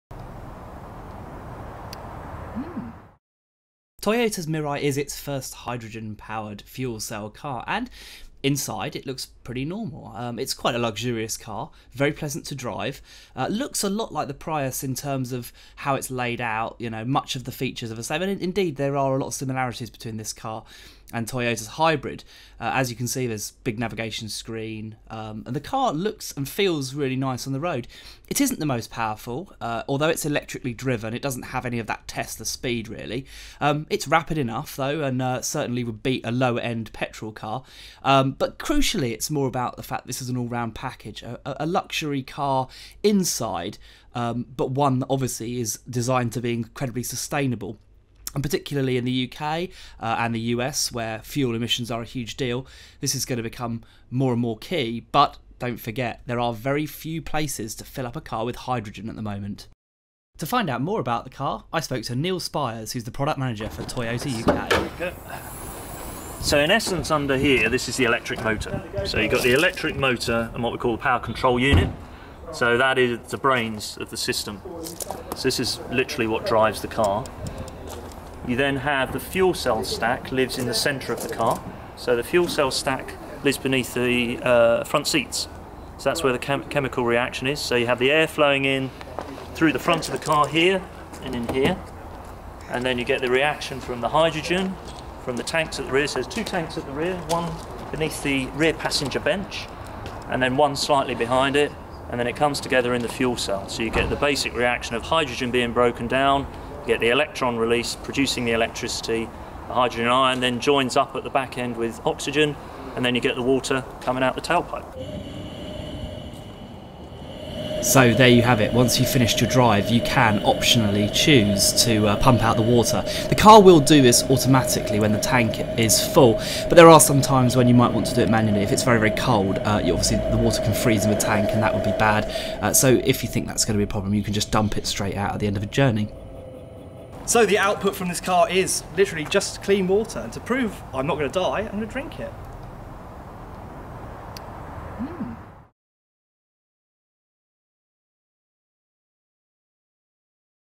DRINKING EXHAUST EMISSIONS from the Toyota Mirai
A hydrogen fuel cell car puts out only water as waste, water that's so clean you can drink it. So I did.